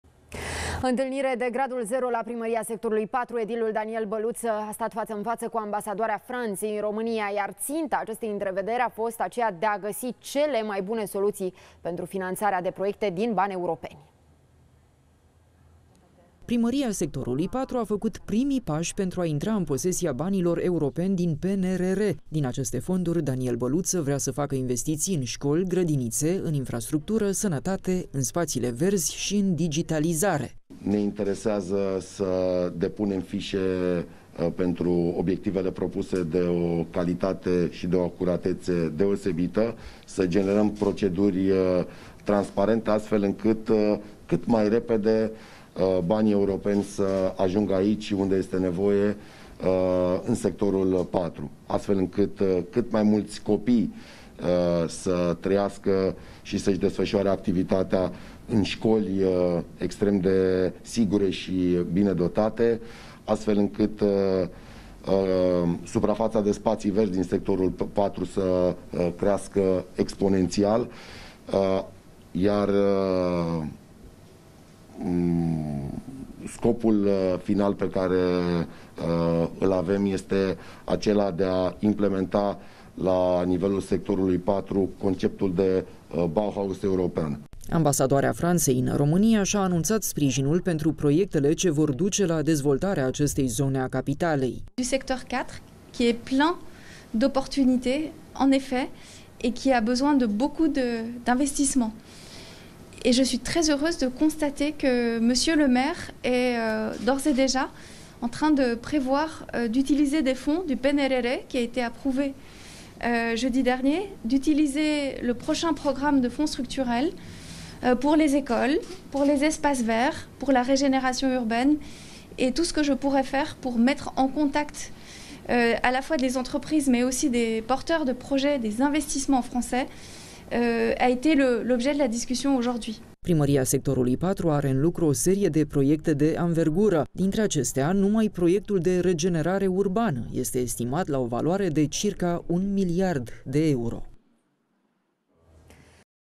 Primăria sectorului 4 face primii paşi pemtru a primi miliardele salvatoare
Întâlnire de gradul zero la Primăria sectorului 4. Edilul Daniel Băluţă a stat faţă în faţă cu ambasadoarea Franţei în România. Ținta întrevederii a fost de a găsi cele mai bune soluții pentru finanţarea de proiecte din bani europeni.